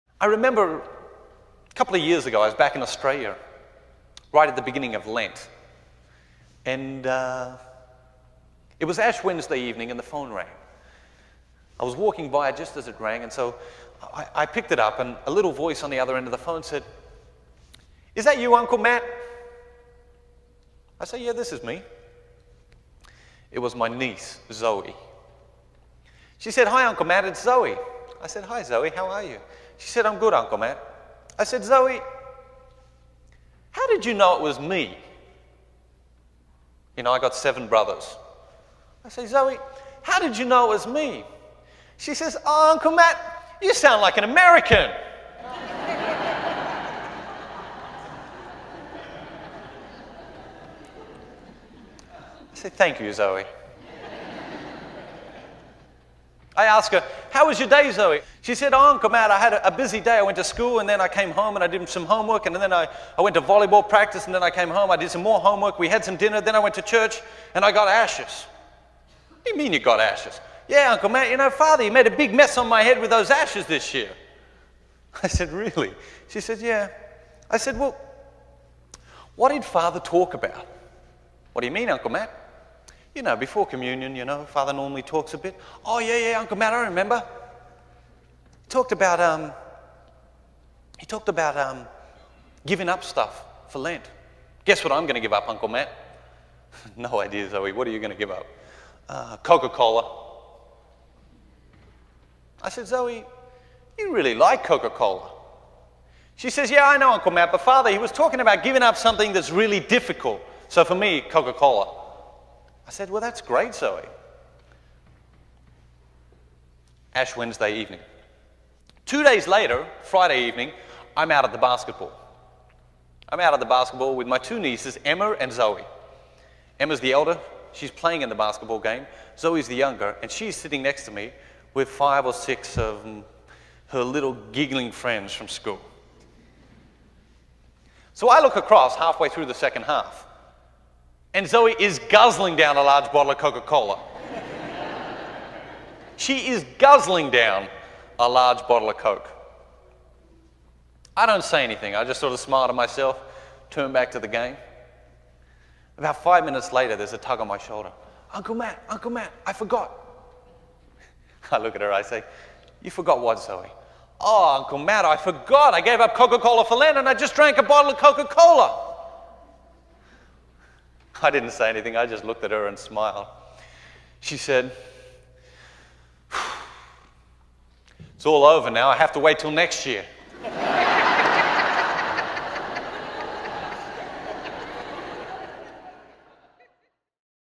What Are You Giving Up for Lent? - Matthew Kelly
Video Transcript:

“I remember, a couple of years ago, I was back in Australia right at the beginning of Lent. It was Ash Wednesday evening and the phone rang. I was walking by just as it rang. And so, I picked it up. And a little voice on the other end of the phone said, "Is that you, Uncle Matt?" I say, "Yeah, this is me." It was my niece, Zoe. She said, "Hi, Uncle Matt, it's Zoe." I said, "Hi, Zoe. How are you?" She said, "I'm good, Uncle Matt." I said, "Zoe, how did you know it was me?" I got seven brothers. I say, "Zoe, how did you know it was me?" She says, "Oh, Uncle Matt, you sound like an American." I said, "Thank you, Zoe." I ask her, "How was your day, Zoe?" She said, "Oh, Uncle Matt, I had a busy day. I went to school, and then I came home, and I did some homework. And then I went to volleyball practice, and then I came home. I did some more homework. We had some dinner. Then I went to church, and I got ashes." "What do you mean you got ashes?" "Yeah, Uncle Matt. Father, he made a big mess on my head with those ashes this year." I said, "Really?" She said, "Yeah." I said, "Well, what did Father talk about?" "What do you mean, Uncle Matt?" "Before communion, Father normally talks a bit." "Oh, yeah, yeah, yeah, Uncle Matt, I remember. He talked about giving up stuff for Lent. Guess what I'm going to give up, Uncle Matt?" "No idea, Zoe. What are you going to give up?" "Coca-Cola." I said, "Zoe, you really like Coca-Cola." She says, "Yeah, I know, Uncle Matt, but Father, he was talking about giving up something that's really difficult. So, for me, Coca-Cola." I said, "Well, that's great, Zoe."

Two days later, Friday evening, I'm out at the basketball. I'm out at the basketball with my two nieces, Emma and Zoe. Emma's the elder. She's playing in the basketball game. Zoe's the younger. And she's sitting next to me with five or six of her little giggling friends from school. So, I look across halfway through the second half, and Zoe is guzzling down a large bottle of Coca-Cola. She is guzzling down a large bottle of Coke. I don't say anything. I just sort of smile to myself. Turned back to the game. About five minutes later, there's a tug on my shoulder, "Uncle Matt, Uncle Matt, I forgot." I look at her, I say, "You forgot what, Zoe?" "Oh, Uncle Matt, I forgot. I gave up Coca-Cola for Lent, and I just drank a bottle of Coca-Cola." I didn't say anything. I just looked at her and smiled. She said, "It's all over now. I have to wait till next year.""